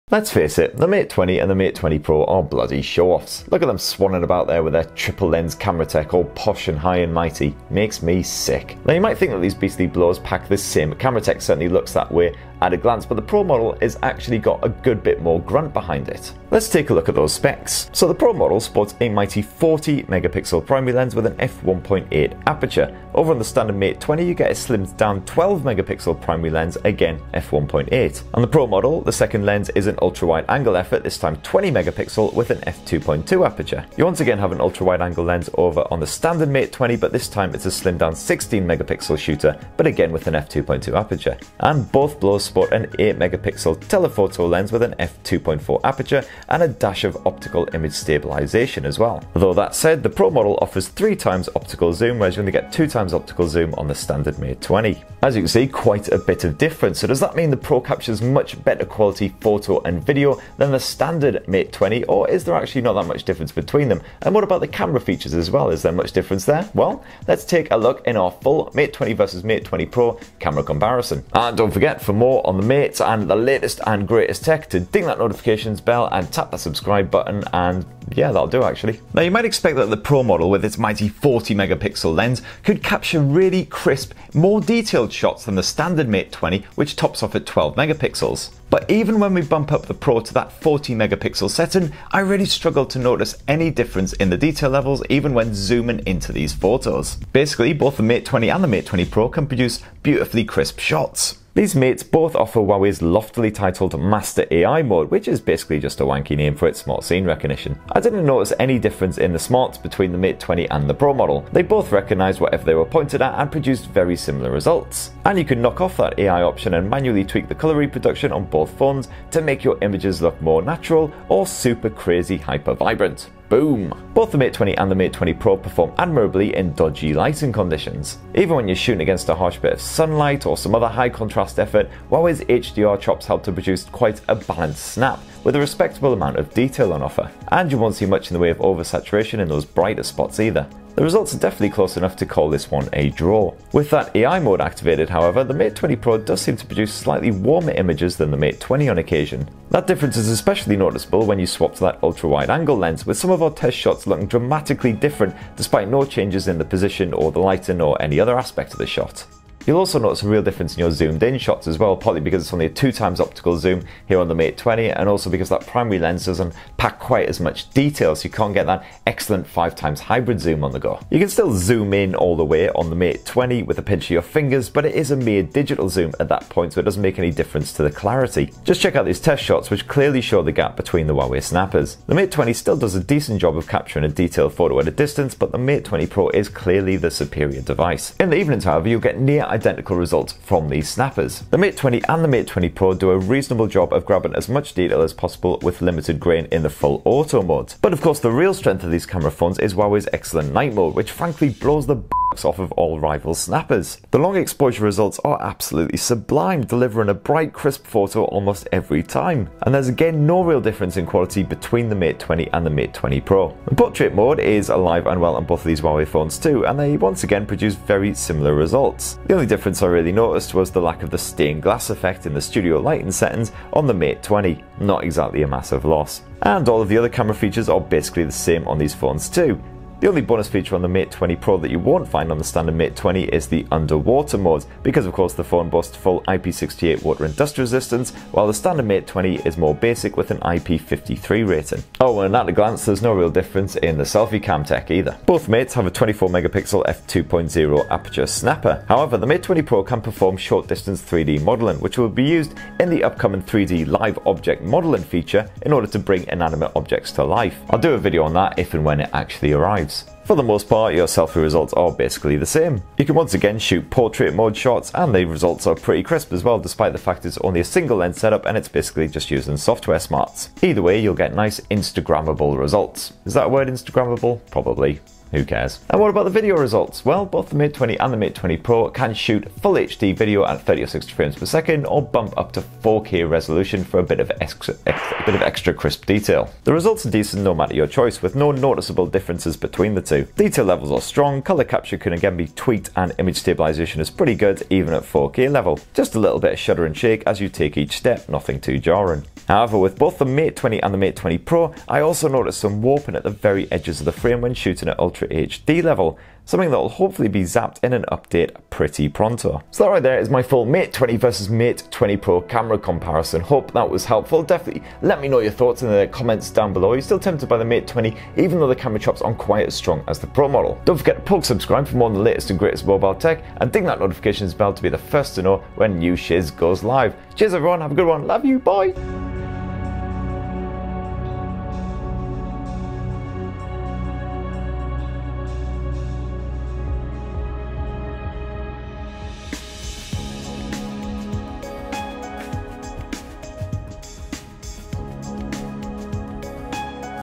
Huawei Mate 20 vs Mate 20 Pro | Camera Comparison
We test and compare Huawei's tri-lens camera on the Mate 20 Pro with the standard Mate 20, to see if there's any difference in photo and video quality and the features on offer. Is the Pro model, with its better-specced camera, the superior of these Huawei phones?

Although they appear to sport the same snappers, there's a clear difference once you glance at the specs. Huawei's Mate 20 Pro model sports a mighty 40MP primary lens, with f/1.8 aperture. The second lens is an ultra-wide-angle effort, this time 20MP, with an f/2.2 aperture. And lastly you have an 8MP telephoto lens with f/2.4 aperture and a dash of Optical Image Stabilisation.

The standard Mate 20 sports a slimmed-down 12MP primary lens, again f/1.8. The ultra-wide-angle is a 16MP shooter this time, again with f/2.2 aperture. And the 8MP telephoto lens is back in action, once again f/2.4 and with OIS in play - although you only get a 2x optical zoom compared with the Mate 20 Pro's 3x optical zoom (and impressive hybrid zoom).

I've been testing the Huawei Mate 20 and Mate 20 Pro cameras side-by-side and here's my full review so you know how the photo and video results differ. Check out my Mate 20 and Pro comparisons with each other, and the best rivals of 2018, including the OnePlus 6T.